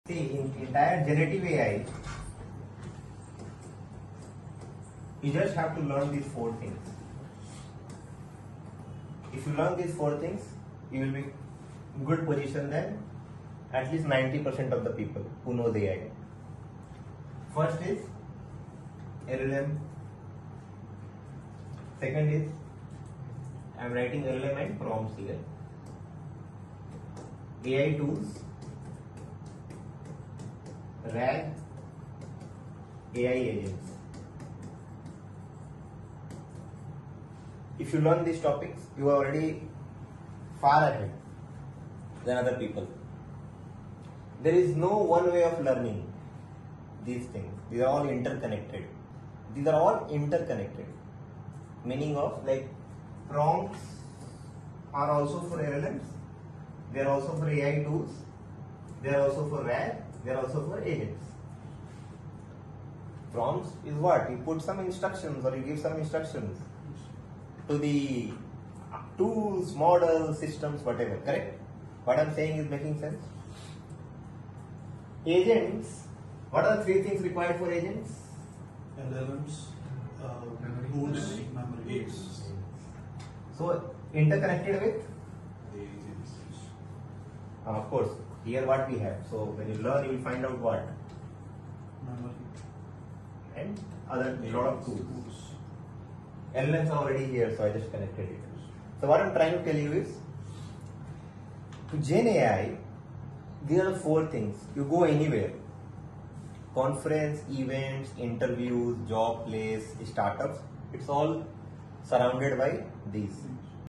4 Skills to Master Generative AI
Unlock GenAI's full potential! Master LLMs, prompts, AI tools, and RAG. These 4 skills put you ahead of 90% of AI users. Learn how they interconnect for rapid innovation.